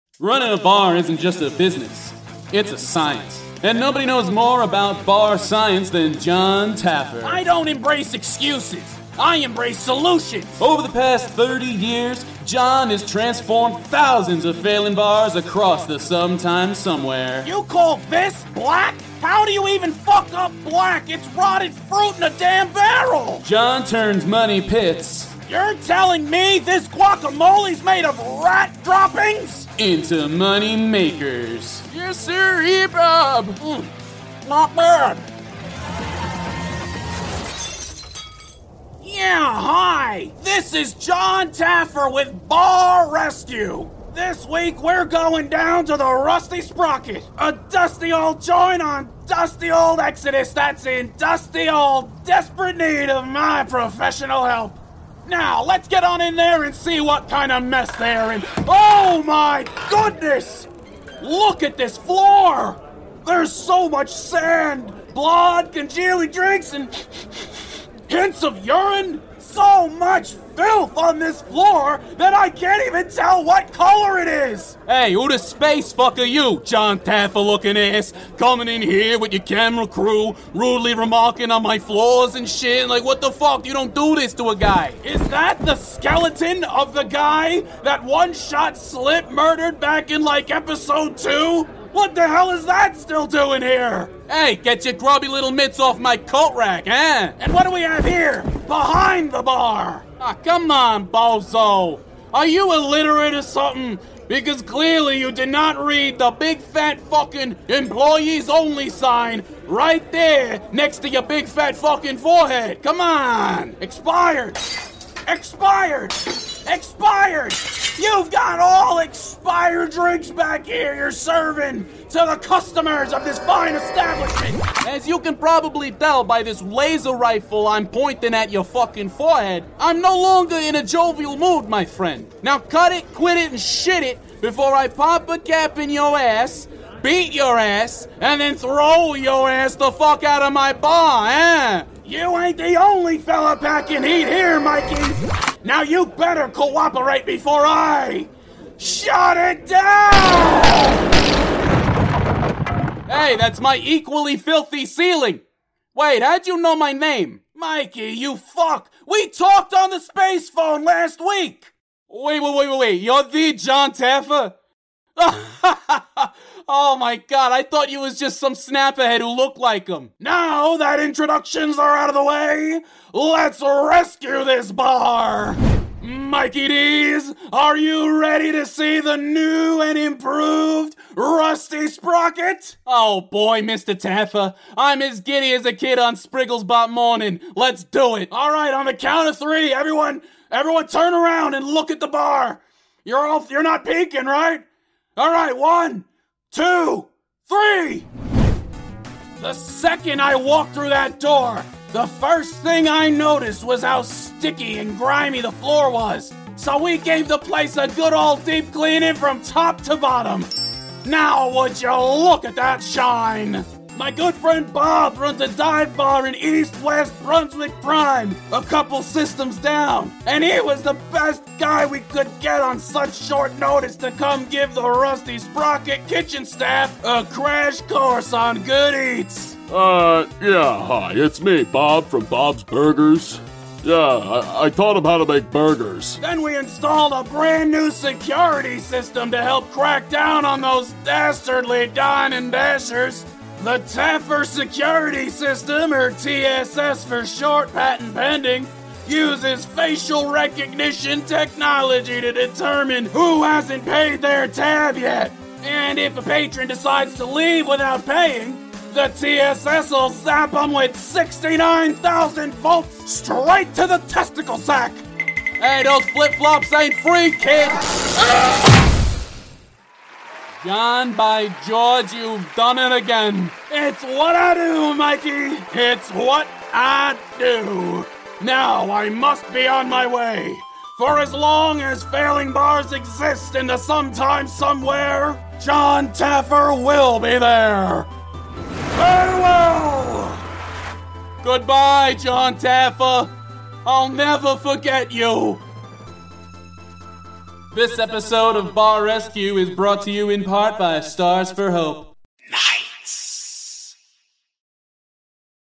Bar Rescue... IN SPACE (Director's Cut)
This week on Bar Resue, Jon Taffer takes a visit to the Rusty Sprocket and teaches everybody a thing or two abour Bar Science...